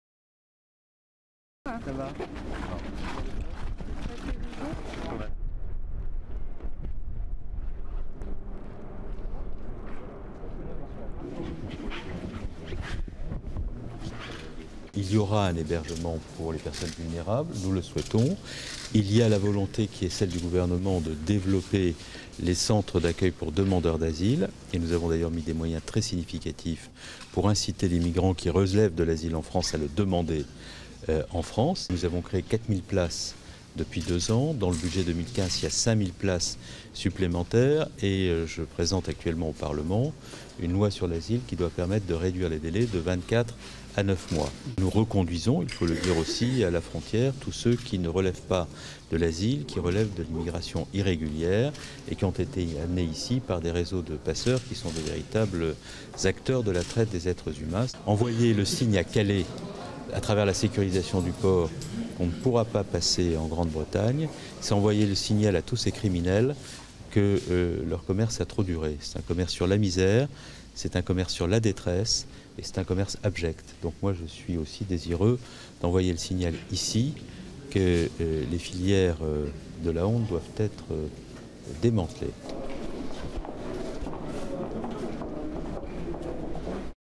Calais: Cazeneuve souhaite "développer les centres d'accueil"
Le ministre de l’Intérieur, Bernard Cazeneuve, a affirmé mercredi 24 décembre à Calais "la volonté du gouvernement de développer les centres d’accueil de demandeurs d’asile". Durée: 01:27